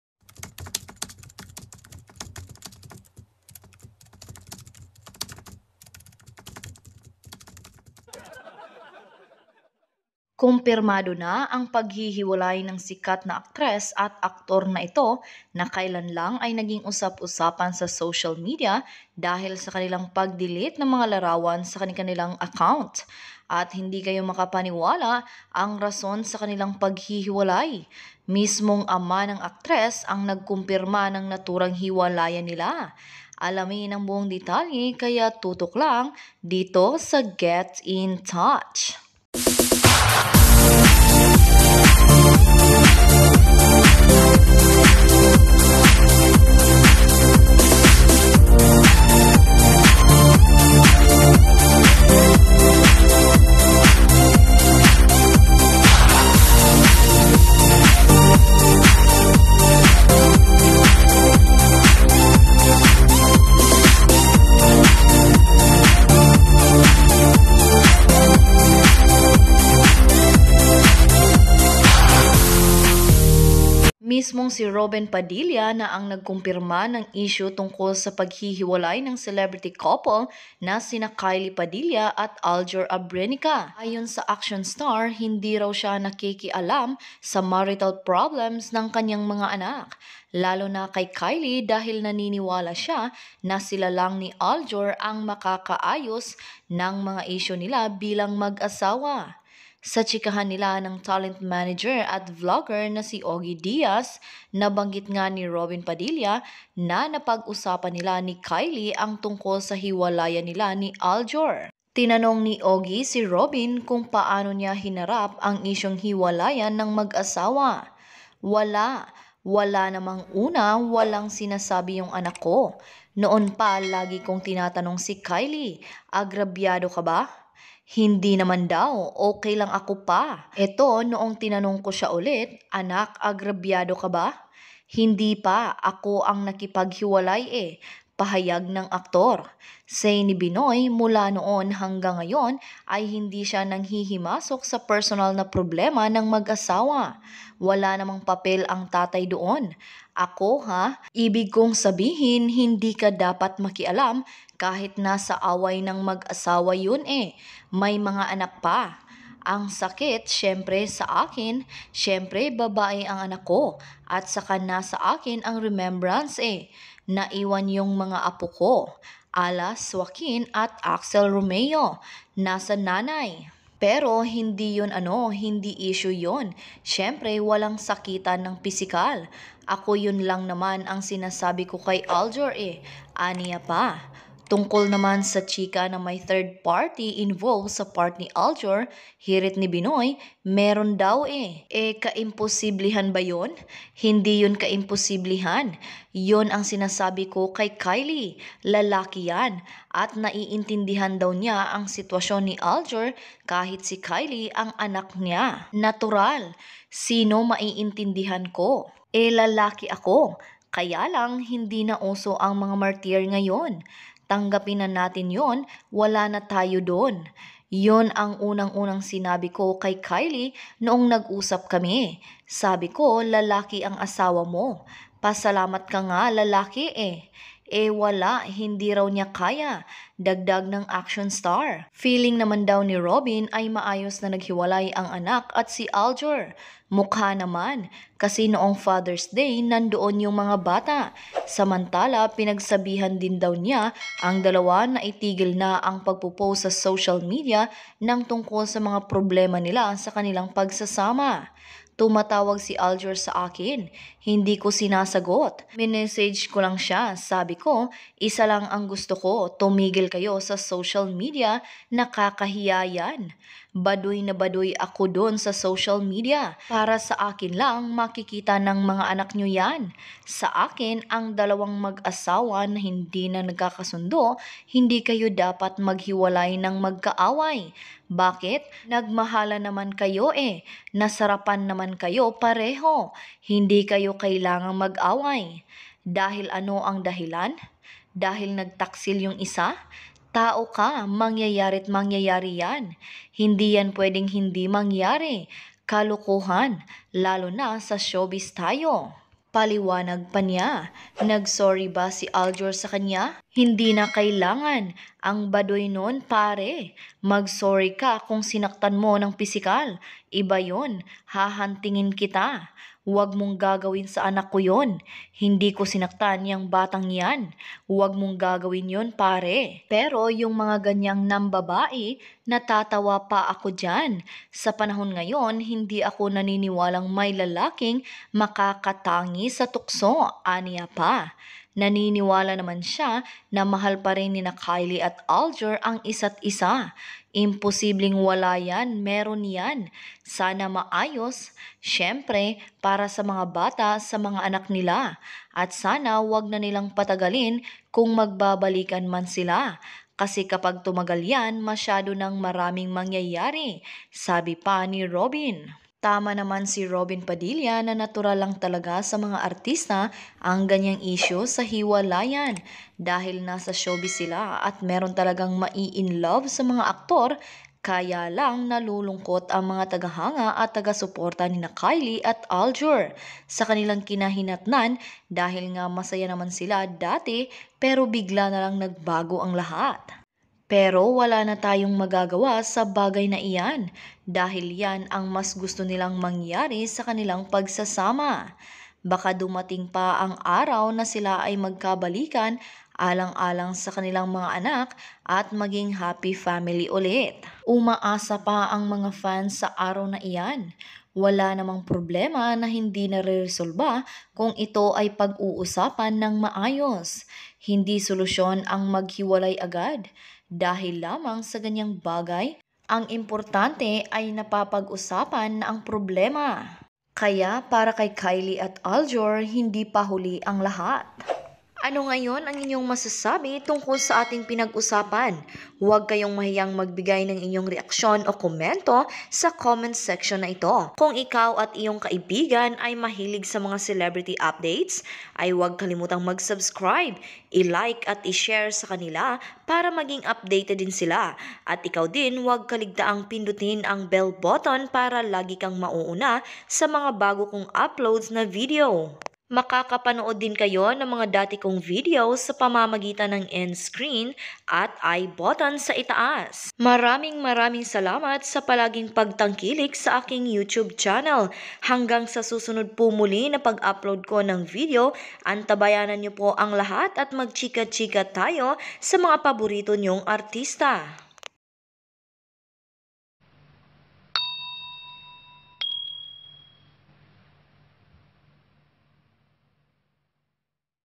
HINDI INASAHAN NG LAHAT ANG DAHILAN NG HIWALAYAN
Hindi inakala ng lahat na ito pala ang tunay na dahilan ng hiwalayan ng sikat na aktor at aktres. Alamin kung ano ito.

At kung gusto niyo ng ibang update tungkol sa mga paborito niyong artista iclick niyo lang ang link na nasa ibaba;